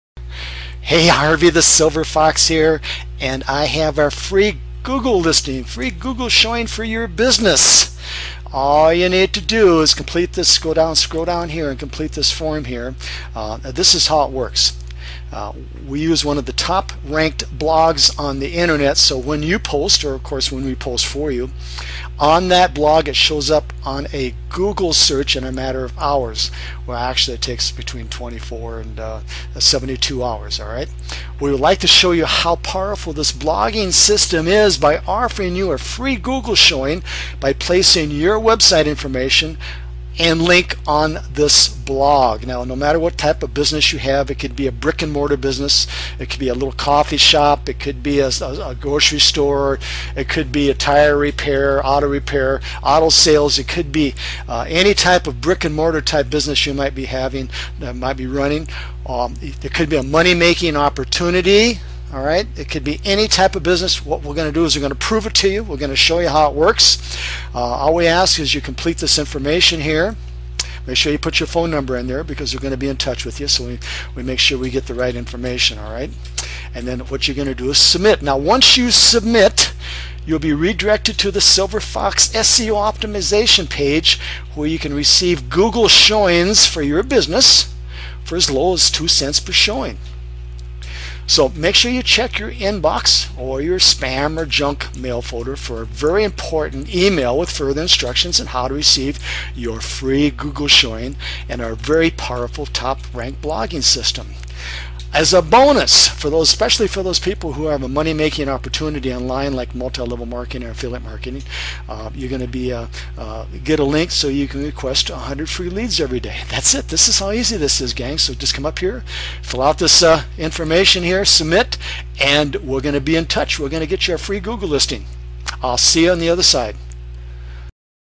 Affiliate Marketing - How to get a free Google Ad and market your link.
By the Affiliate Marketing Guru, Harvey the Silver Fox